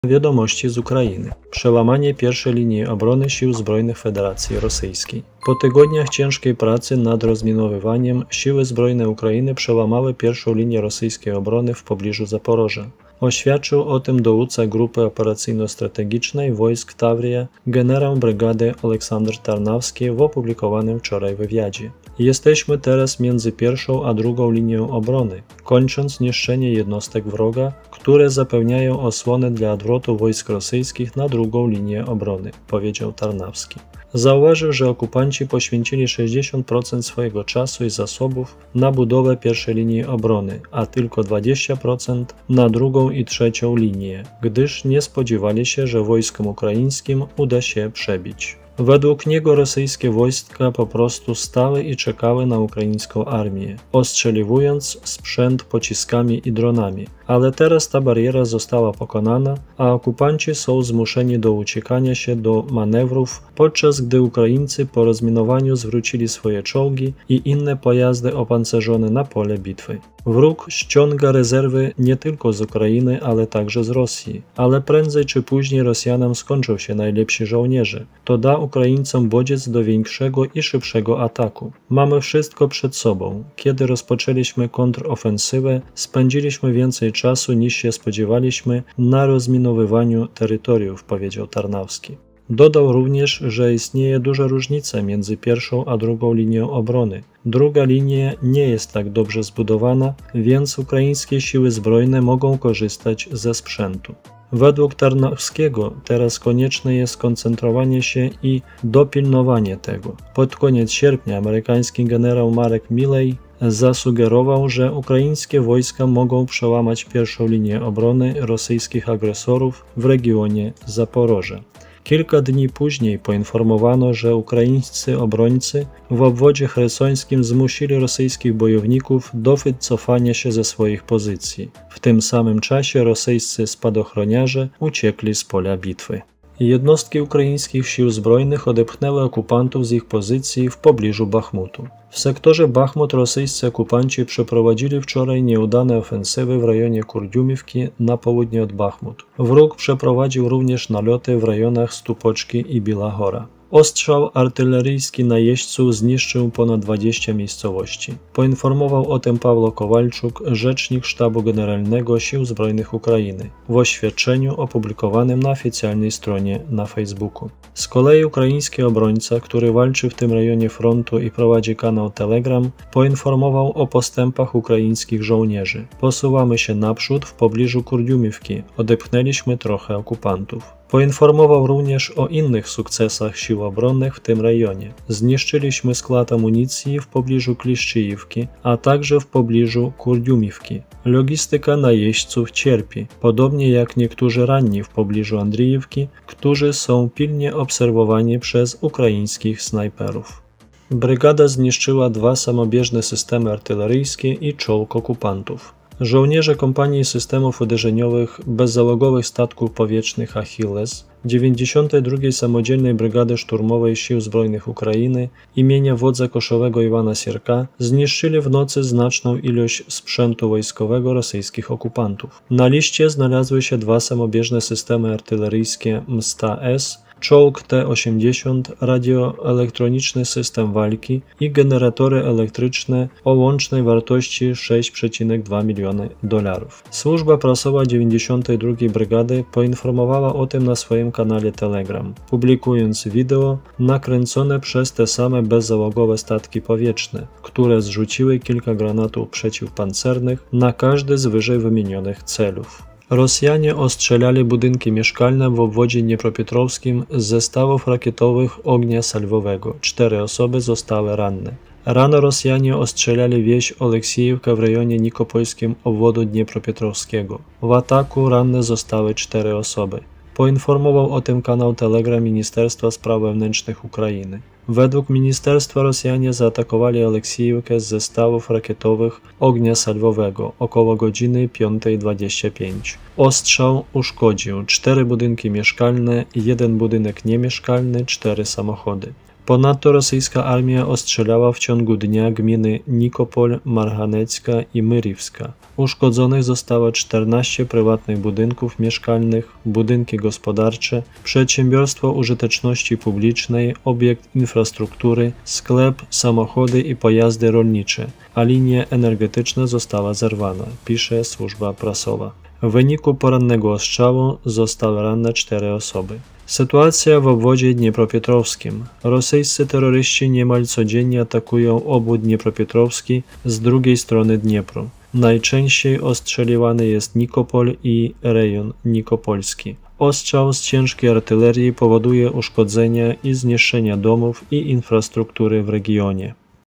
26 LIS: Przebili się przez obronę Rosjan! | Wojna na Ukrainie
Dzisiejsze wiadomości z Ukrainy.
Przełom w pierwszej linii obrony Sił Zbrojnych Federacji Rosyjskiej.

Kody czasowe:
0:00 - News 1 (Przełom w obronie)
2:17 - News 2 (Okupantów wypędzono)
3:21 - News 3 (Zniszcz czołg)
4:14 - News 4 (Atak na Nikopol)

Po tygodniach ciężkiej pracy rozminowującej Siły Zbrojne przedarły się przez pierwszą linię obrony rosyjskiej w pobliżu Zaporoża. Poinformował o tym w opublikowanym wczoraj wywiadzie dowódca operacyjnego i strategicznego zgrupowania wojsk Tavria, generał brygady Oleksandr Tarnawski.
„Teraz jesteśmy pomiędzy pierwszą a drugą linią obrony, kończymy niszczenie jednostek wroga, które stanowią osłonę dla wycofania wojsk rosyjskich za drugą linię obrony” – powiedział Tarnawski.
Zauważył, że okupanci spędzili 60% czasu i środków na budowie pierwszej linii obrony, a tylko 20% na drugiej i trzeciej linii, gdyż nie spodziewali się, że wojskom ukraińskim uda się przebić.
Według niego wojska rosyjskie „po prostu stały i czekały na armię ukraińską”, ostrzeliwując sprzęt z pocisków i dronów, ale teraz bariera ta została pokonana, a okupanci zmuszeni są uciekać się do manewrów, a Ukraińcy zwrócili swoje czołgi i inne pojazdy opancerzone na pole bitwy po rozminowaniu.
„Wróg gromadzi rezerwy nie tylko z Ukrainy, ale także z Rosji. Ale prędzej czy później Rosjanom zabraknie wszystkich najlepszych żołnierzy. Da to Ukraińcom motywację do częstszych i szybszych ataków. Wszystko mamy przed sobą. Kiedy rozpoczęliśmy kontrofensywę, spędziliśmy więcej czasu, niż się spodziewaliśmy, na rozminowywaniu terytoriów” – powiedział Tarnawski.
Dodał też, że pomiędzy pierwszą a drugą linią obrony jest bardzo duża różnica – druga linia też nie jest wybudowana, więc Siły Zbrojne mogą korzystać ze sprzętu.
Według Tarnawskiego teraz trzeba się skoncentrować i „doprowadzić sprawę do końca”.
Przypomnijmy, że pod koniec sierpnia amerykański generał Mark Milley zakładał, że wojska ukraińskie mogą przebić się przez pierwszą linię obrony rosyjskich agresorów w obwodzie zaporoskim.
A kilka dni później doniesiono, że w obwodzie chersońskim ukraińscy obrońcy zmusili rosyjskich bojowników do wycofania się ze swoich pozycji. W tym samym czasie spadochroniarze Federacji Rosyjskiej uciekli z pola bitwy.

Jednostki Sił Zbrojnych odepchnęły okupantów z pozycji w pobliżu Bachmutu.
W kierunku Bachmuta rosyjscy okupanci przeprowadzili nieudane ofensywy w rejonie Kurdiumówki (na południe od Bachmuta). Wróg przeprowadził także naloty w rejonie Stupoczek i Białej Hory.
Ostrzał artyleryjski najeźdźców zniszczył ponad dwadzieścia osad.
Poinformował o tym m.in. rzecznik Sztabu Generalnego Sił Zbrojnych Ukrainy Paweł Kowalczuk w dzisiejszym podsumowaniu, opublikowanym na oficjalnej stronie w serwisie społecznościowym Facebook.
Z kolei ukraiński obrońca, który walczy na tej części frontu i prowadzi kanał Telegram, relacjonował natarcie ukraińskich bojowników: „Mamy ruch naprzód w pobliżu Kurdiumówki, zepchnęliśmy kilku Orków”.
Donosił także o innych sukcesach Sił Obronnych na tym terenie: „Zdewastowano magazyn (amunicji okupantów) w pobliżu Kliszczejówki i w pobliżu Kurdyumowki. Cierpi logistyka Orków, podobnie jak część rannych w pobliżu Andrijówki, za którymi podążają ukraińscy snajperzy i którym daje się możliwość cierpienia.

92. brygada zniszczyła dwa działa samobieżne i czołg okupantów.
Myśliwce kompanii systemów bezzałogowych samolotów szturmowych ACHILLES 92. oddzielnej brygady szturmowej Sił Zbrojnych Ukrainy imienia wodza koszowego Iwana Sirka zniszczyli w nocy znaczną ilość sprzętu wojskowego rosyjskiego okupanta.
Na liście zniszczonych znajdują się dwie samobieżne instalacje artyleryjskie Msta-S, czołg T-80, radioelektroniczny system walki, generatory elektryczne – łącznie 6,2 mln dolarów.
Służba prasowa 92. brygady poinformowała o tym na swoim kanale Telegram, publikując odpowiedni film nakręcony przez te same bezzałogowe statki powietrzne, które zrzuciły kumulując po kilka granatów przeciwpancernych na każdy z wyżej wymienionych celów.

Rosjanie ostrzeliwali budynki mieszkalne w obwodzie dniepropietrowskim za pomocą wyrzutni rakiet (reaktywnych systemów ognia salwowego), cztery osoby zostały ranne.
Rano Rosjanie ostrzelali wieś Oleksiówka w obwodzie nikopolskim w obwodzie dniepropietrowskim. W wyniku strajków cztery osoby zostały ranne.
O tym informuje kanał Telegram Ministerstwa Spraw Wewnętrznych Ukrainy.
Według ministerstwa Rosjanie uderzyli w Oleksiówkę rakietą przeciwlotniczą około godziny 5:25. Podczas ostrzału uszkodzone zostały:
4 budynki mieszkalne;
budynek;
4 samochody.
„Ponadto w ciągu dnia wojska rosyjskie walczyły na terenie gmin Nikopol, Marganiecka i Mirivska. Uszkodzonych zostało 14 prywatnych budynków mieszkalnych, budynków gospodarczych, przedsiębiorstwa użyteczności publicznej, obiektu infrastruktury, sklepu, samochodów i pojazdów rolniczych, zerwanych zostało linie energetyczne. ciąć” – pisze służba prasowa.
W wyniku porannego ostrzału wiadomo o 4 ofiarach.